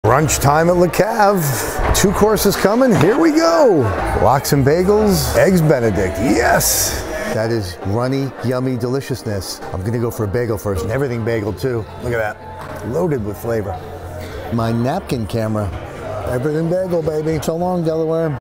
Bring on the brunch - Le Cavalier in Wilmington Delaware
I traveled to 14 states in a 2 month period this summer discovering the best restaurants in every state in the northeast and a few from the southeast. I was pleasantly surprised with the quality restaurants I ate at in Wilmington Delaware. If you're like me you've driven through Delaware but have never stopped to admire it.  It's a wonderful state and they're serving up some lovely food..